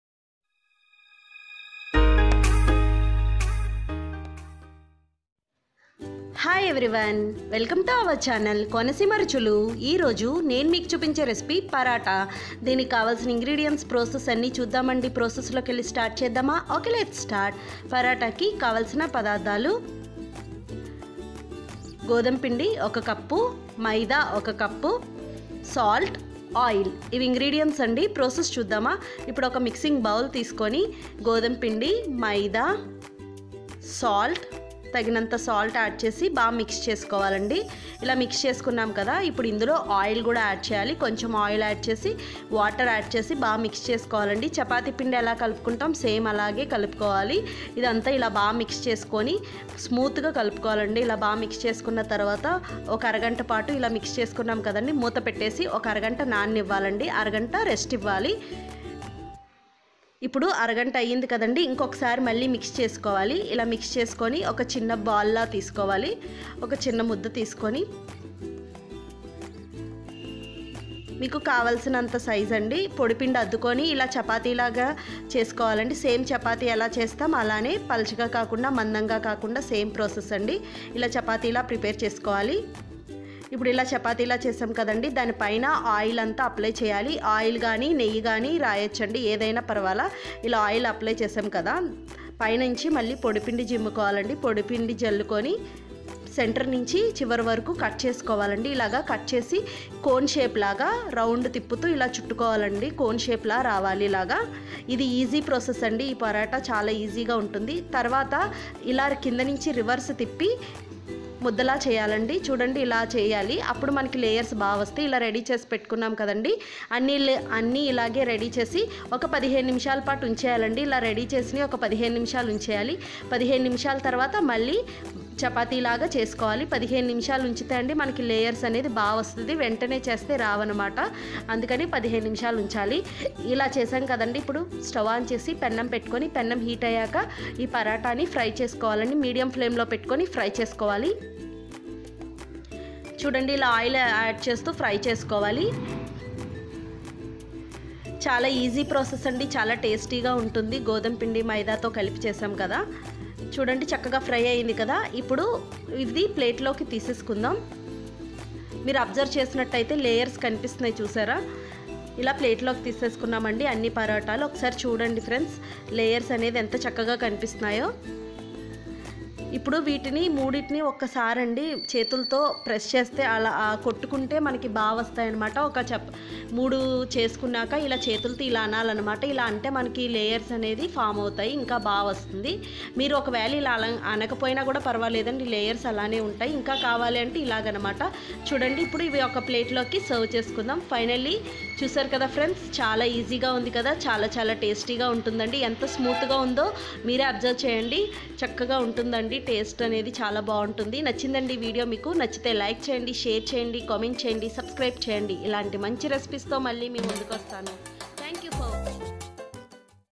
చాలా సింపుల్ ప్రోసెస్ తో పరాటాలు ఇలా చేసి చూడండి||How To Make Simple Process Soft Layered Paratha
Tasty tasty paratha

How to make soft leyered paratha recipe

Esay and simple process paratha recipe